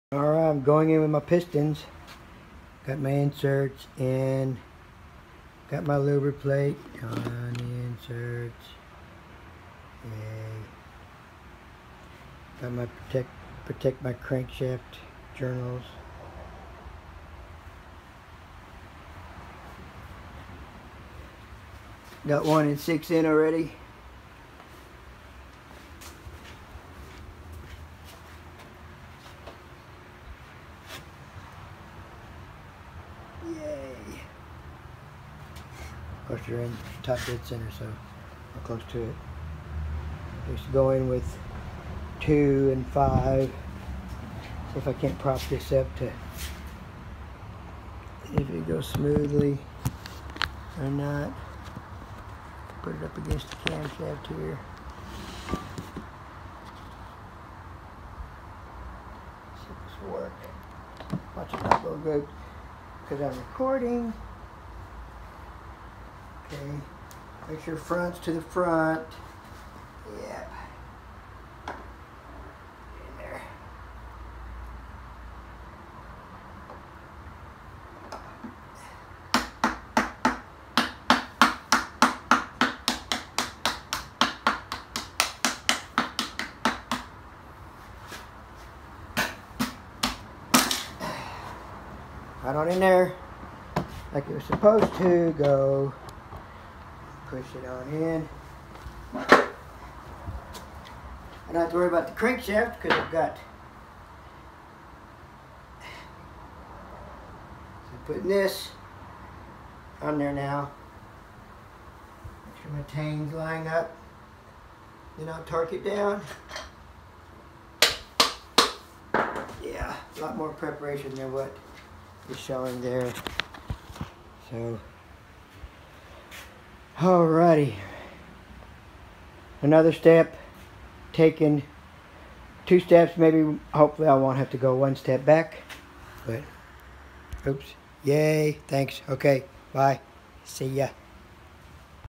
8.3 Liter Cummins C-Series assembly.
Going in with the pistons on my 8.3 l Cummings C series diesel.